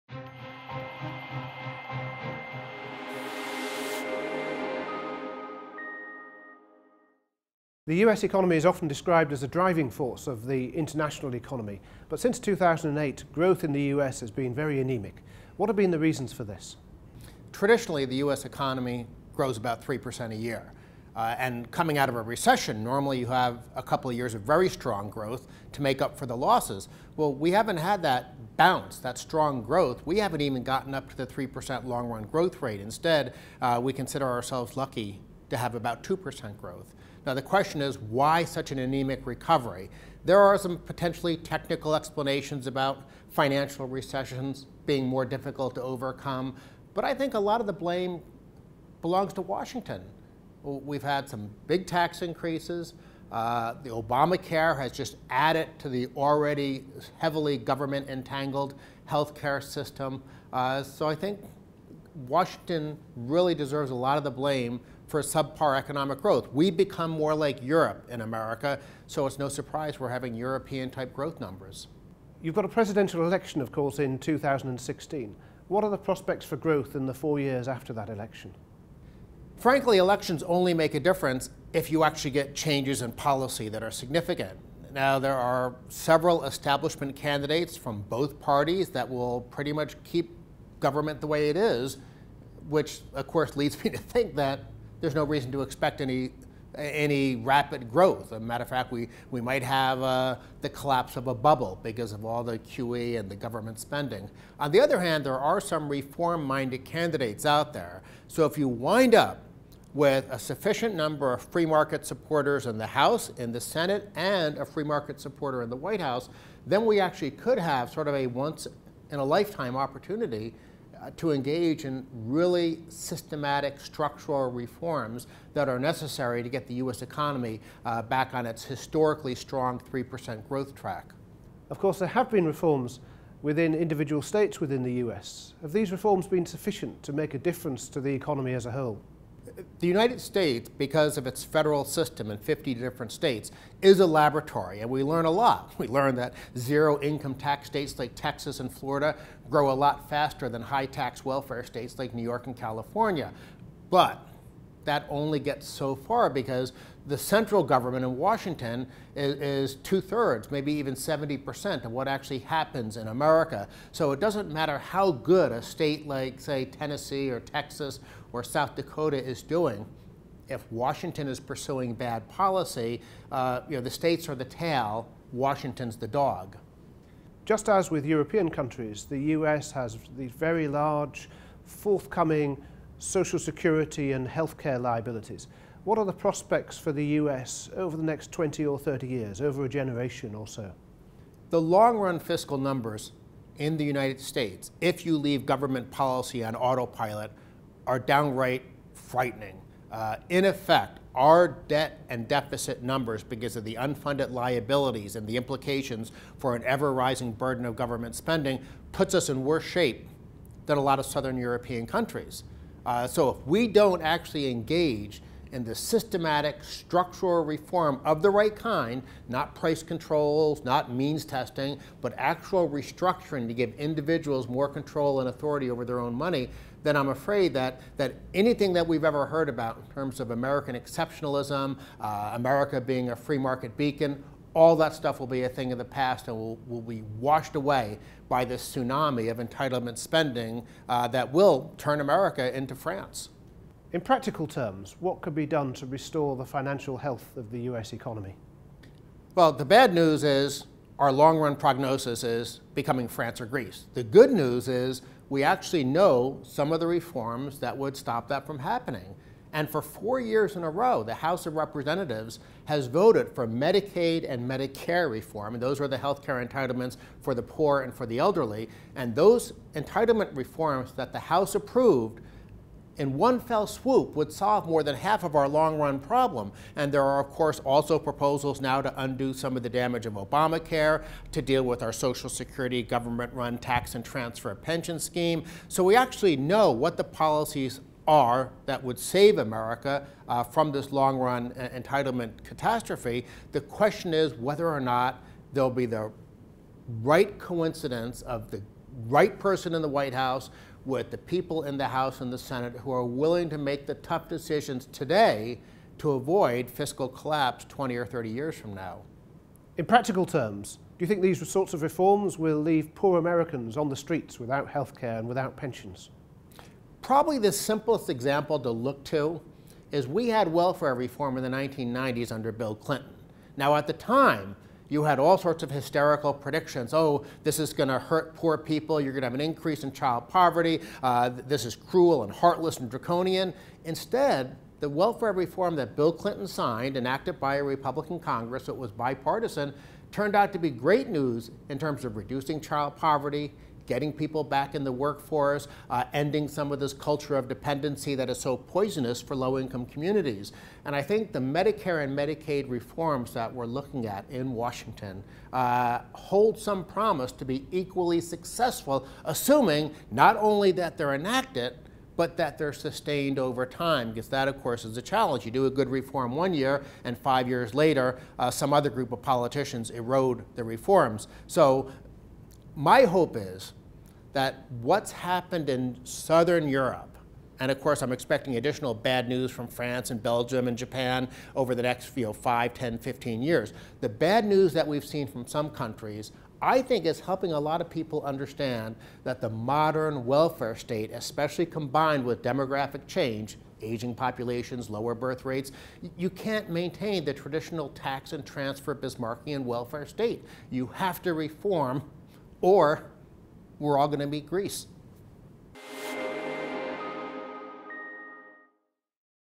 Dan Mitchell interview
Cato Institute Senior Fellow Dan Mitchell speaks to ieaTV following the State of the Economy Conference